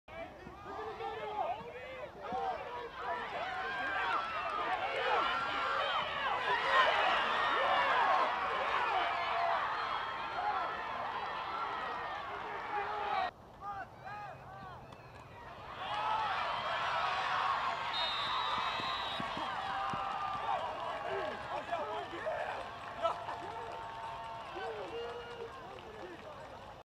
Friday Night Frenzy Week 3: North Mecklenburg HS vs. Harding University HS
Who won during this clash on the gridiron for Week 3 of high school football?